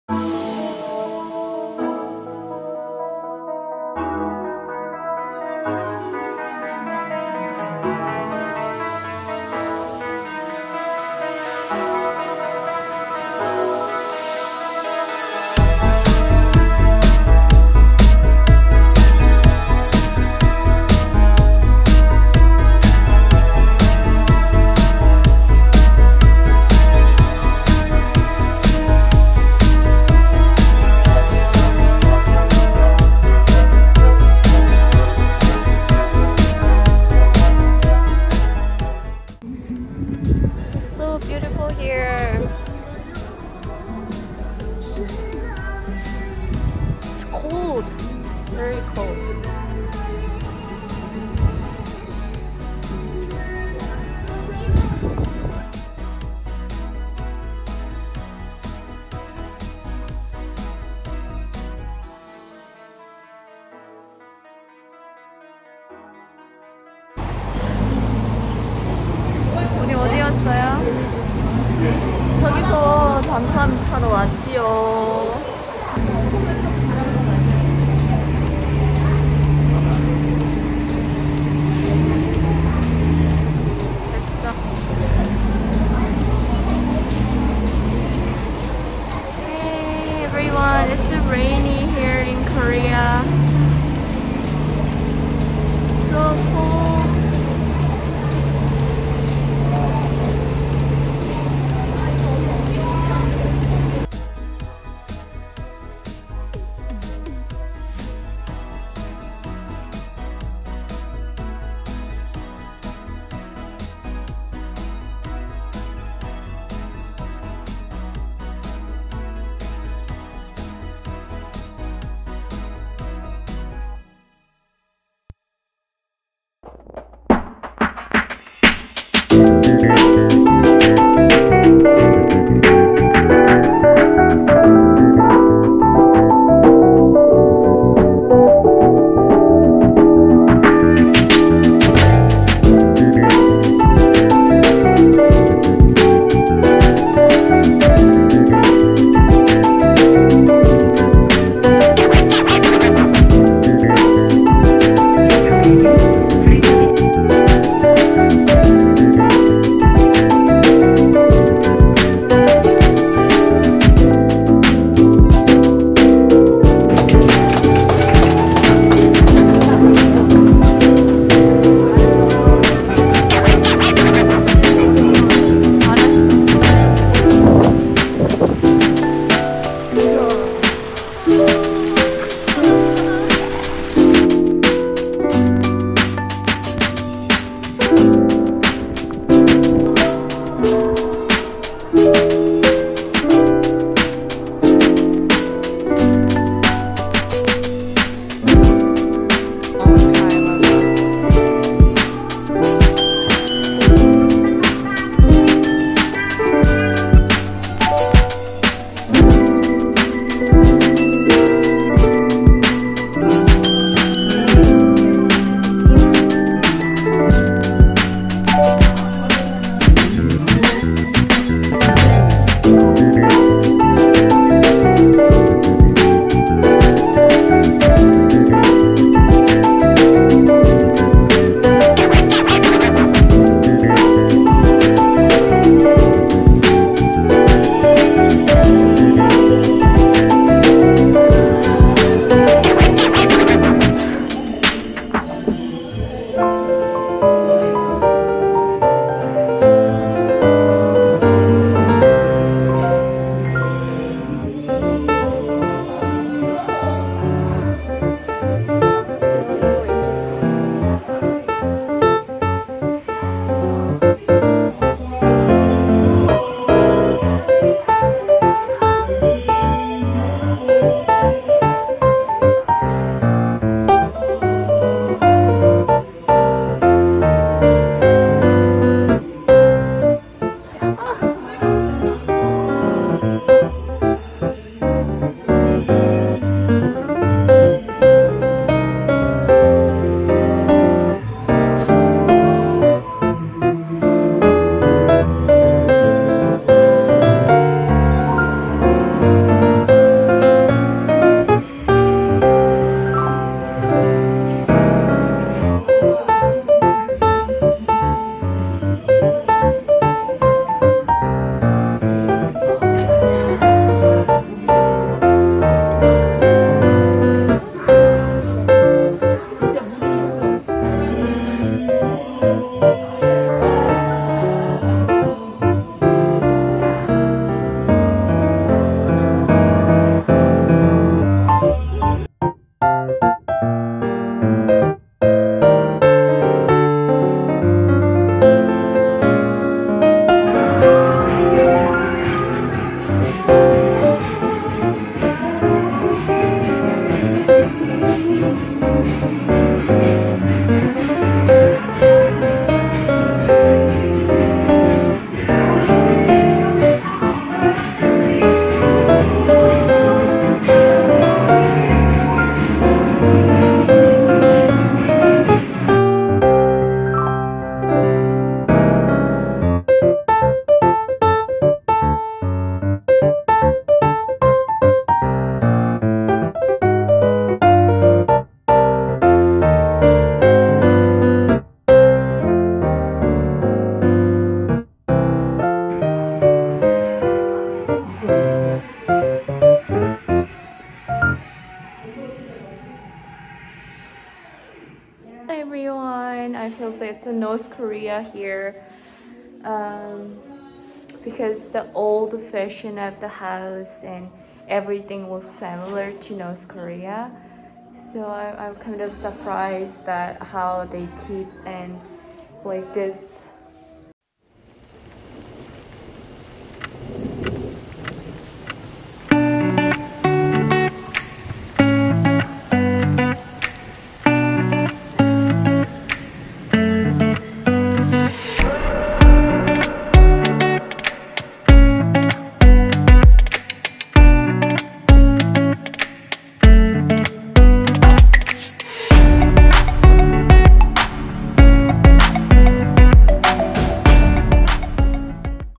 Jeju island with my North Korean family. 제주도 가족 여행.
This is my first time with my family in South Korea to visit them. We were so excited about Jeju island but I slept too much in the nice hotel room. Hahah Anyways I hope you all enjoy my videos.

I will keep post my vides stay with me!
Thank you everybody again.